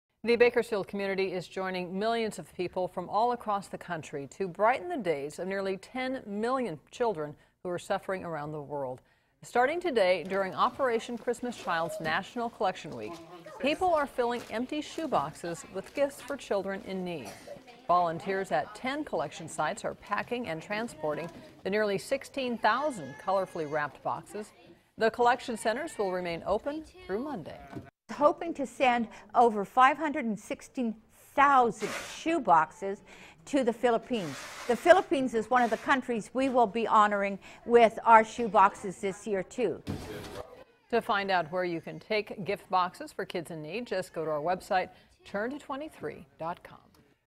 Operation Christmas Child in Bakersfield starts annual collection
Volunteers help collect 15,750 gifts for children in need.